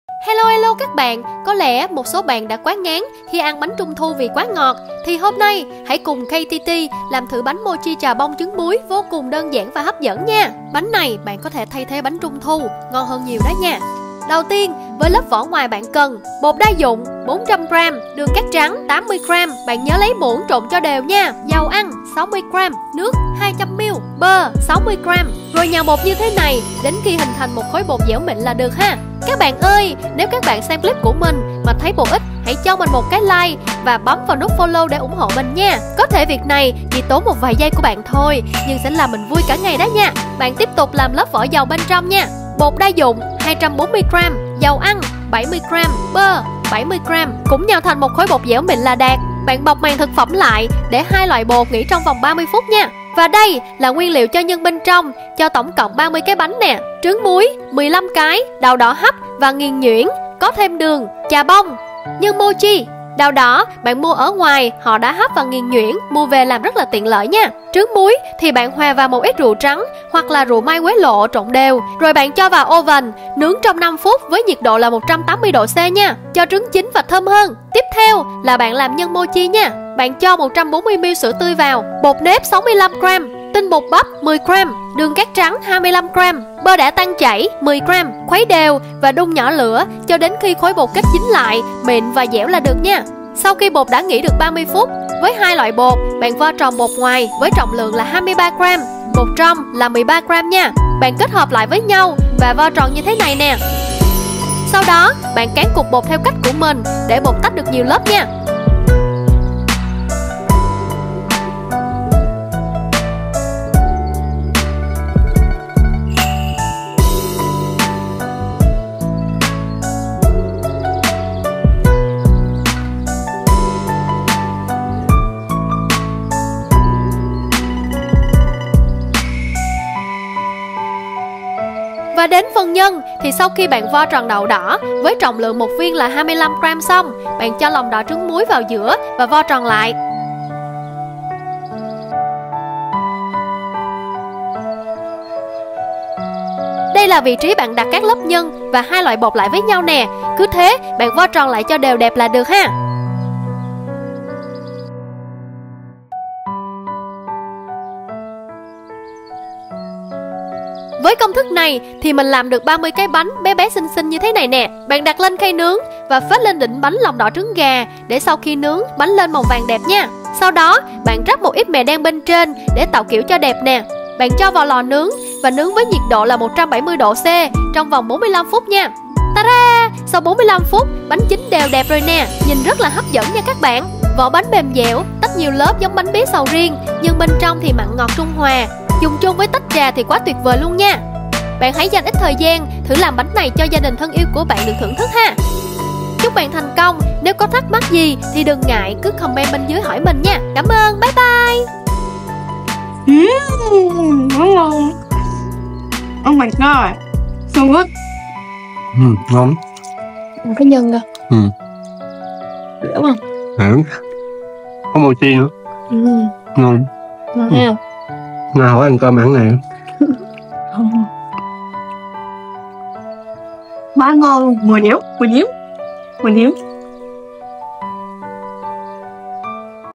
Cách Làm Bánh Mochi Chà Bông Trứng Muối I How to bake Pork Floss Mochi Cake with Salted Egg
Pork Floss Mochi with Salted Egg Cake Recipe :

Inside Filling:
- Salted egg: 15pcs
- Red Bean Paste: 750g
- Pork Floss : 100g
- Mochi : 300g

Inside Dough:
- All purpose flour: 240g
- Butter: 70g
- Oil: 70g

Outside Dough:
- All purpose flour: 400g
- Butter: 60g
- Oil: 60g
- White sugar: 80g
- Water: 200g

Mochi:
- Glutinous rice flour: 65g
- Cornstarch: 10g
- White sugar: 25g
- Milk: 140g
- Melted butter:10g